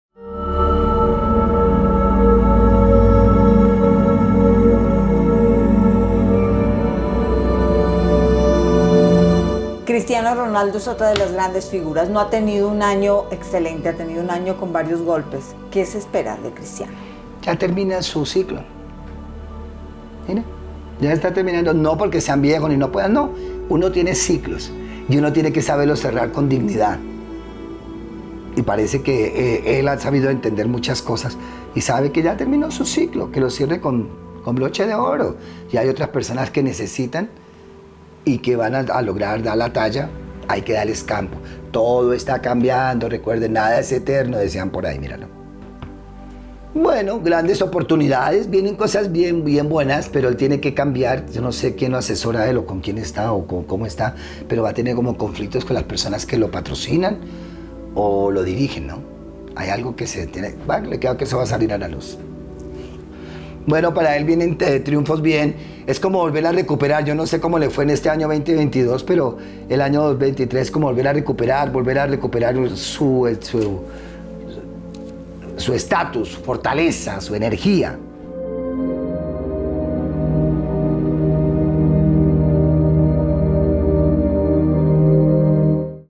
Predicciones 2023, para las estrellas del deporte destacados con el profesor Salomón
¿Cómo será el año 2023 para Cristiano Ronaldo, Egan Bernal  y Nairo Quintana?